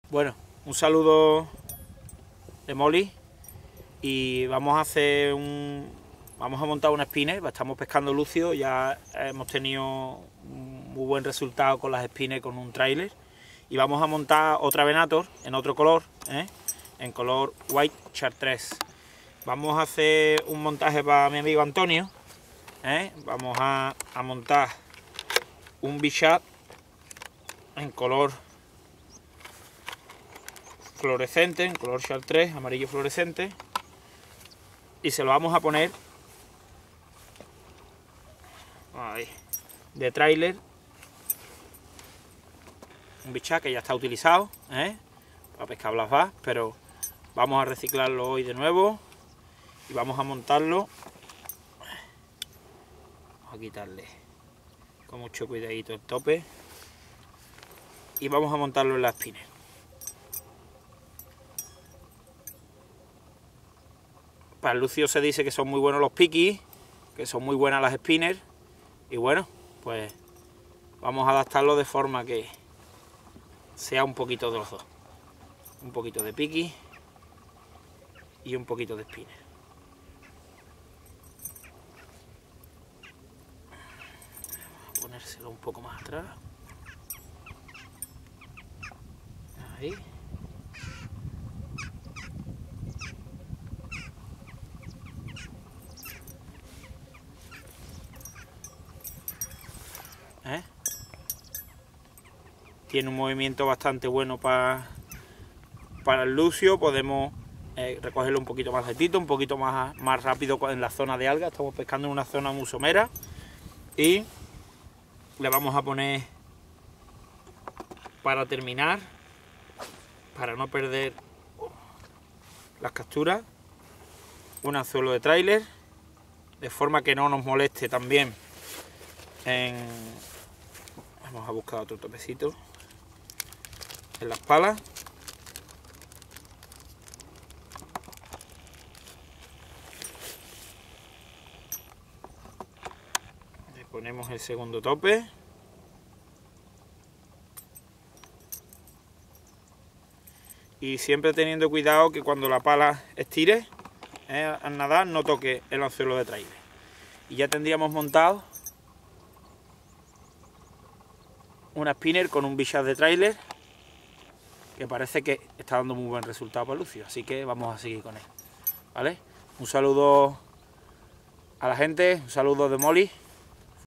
Humberto Gaccio , montaje spinner para lucio. (LeaderPesca)
Mas informacion en  Leaderpesca.com y en el Tfno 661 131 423.
Humberto gaccio nuestro amigo y gran pescador nos desvela en leaderpesca un secreto para

engañar a esos grandes lucios extremeños.